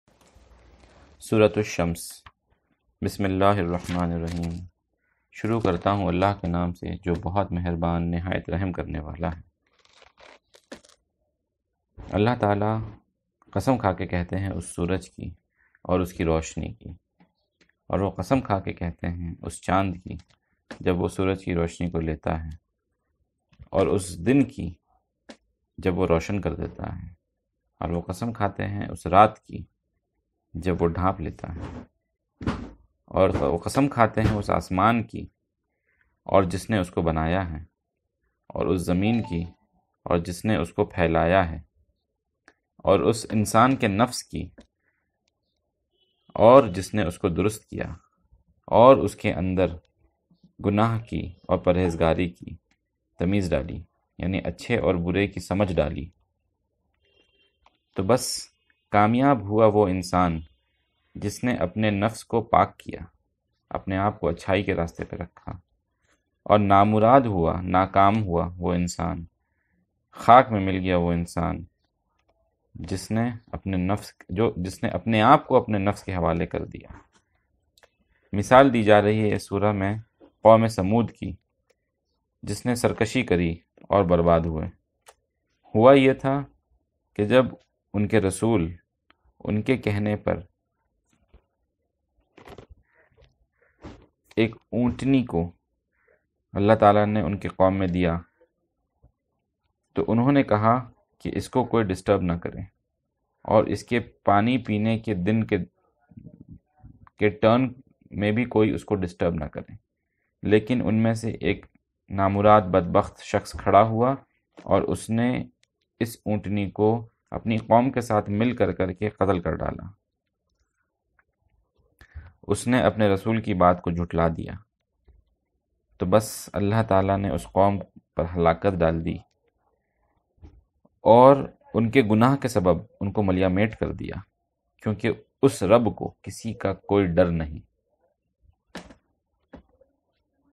Meaningful Tarjuma of 91st Surah Ash Shams (full) of The Holy Quran in Urdu Hindi
Urdu Hindi Tarjuma of Surah Ash Shams the 91st chapter of The Holy Quran in the soothing voice of Atif Adnan.

Quran tilawat and recitation in the sweet and beautiful voice with Tajweed. Also showing the fazeelat and translation at the end of the beautiful recitation.

Best Tarjuma of Quran Recitation - Emotional Soft Quran Recitation - Best Quran Listen in World